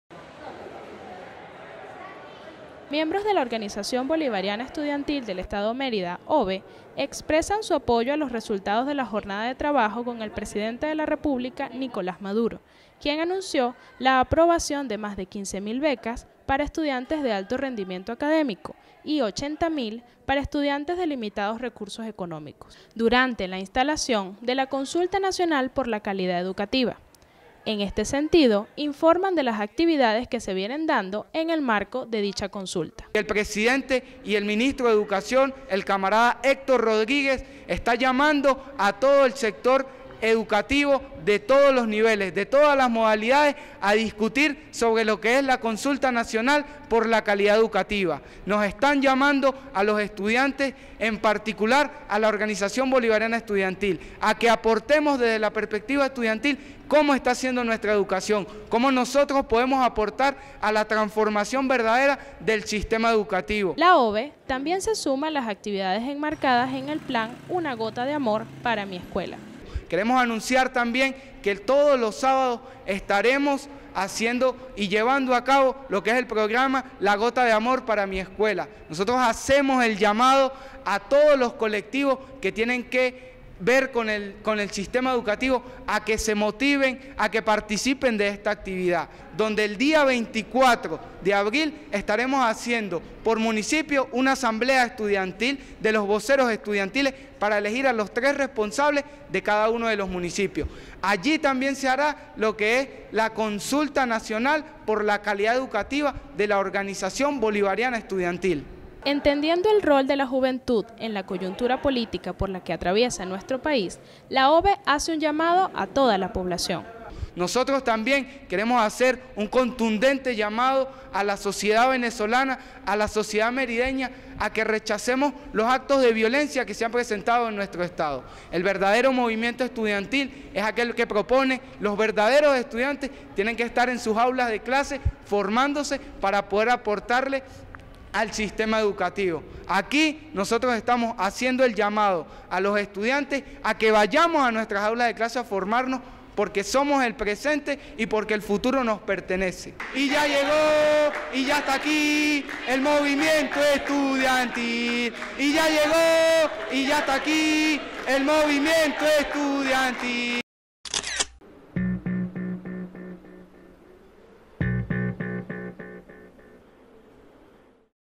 La OBE se suma a la Consulta Nacional por la Calidad Educativa
Miembros de la Organización Bolivariana Estudiantil del Estado Mérida OBE, expresan su apoyo a los resultados de la jornada de trabajo con el Presidente de la República Nicolás Maduro,  quien anunciara la aprobación de más de quince mil becas para estudiantes de alto rendimiento académico y 80 mil para estudiantes de limitados recursos económicos, durante la instalación de la "Consulta Nacional por la Calidad Educativa" el pasado 28 de marzo de 2014, en este sentido, informan de las actividades que se vienen dando en el marco de dicha consulta.